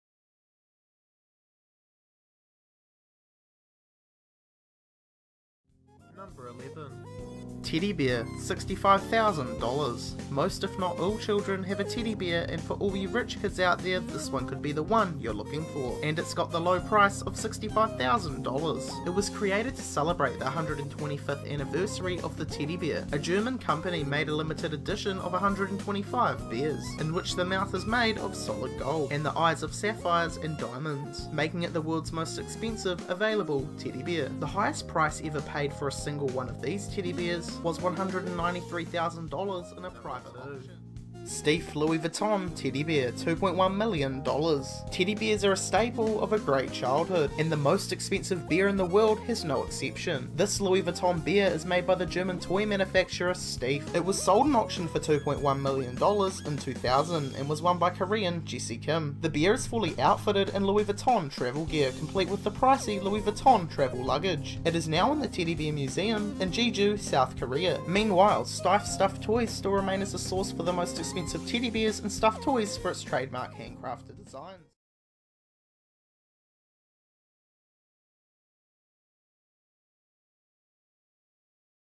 Two VERY expensive teddy bears
For those out there that complain that plushies cost too much, you haven't seen NOTHING! Check out the prices for these two teddy bears!